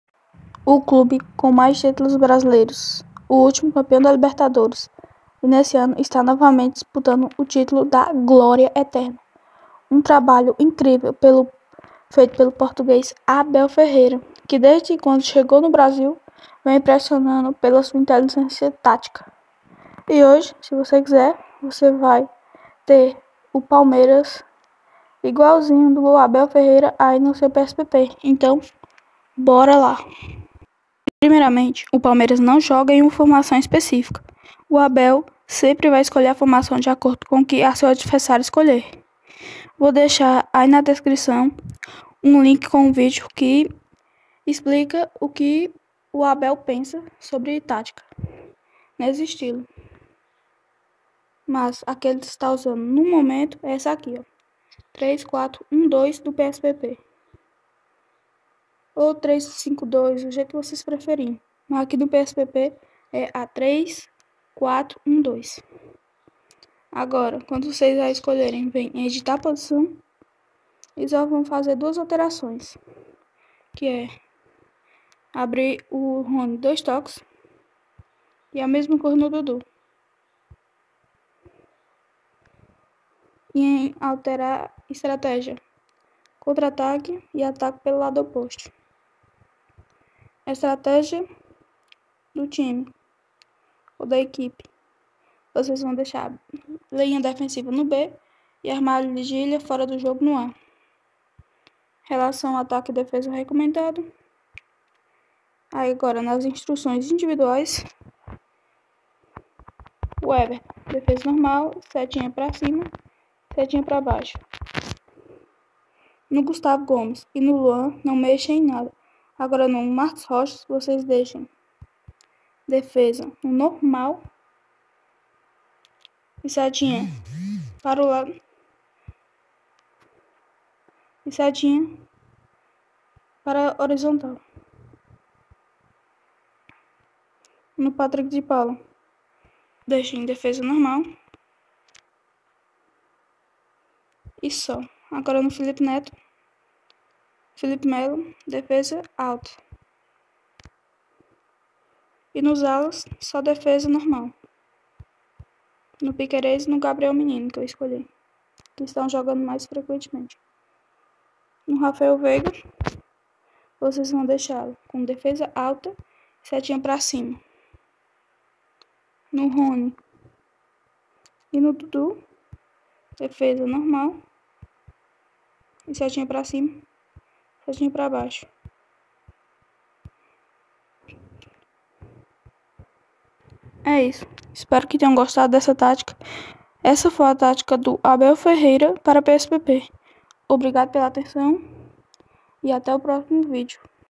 AVANTI PALESTRA!!! Aprenda a jogar exatamente como o Palmeiras do Abel Ferreira | PPSSPP
O LINK DO VIDEO
SOBRE O PENSAMENTO TATICO
DO ABEL FERREIRA 👇👇👇👇

E ISSO AVANTI PALESTRA!!!🐷🐷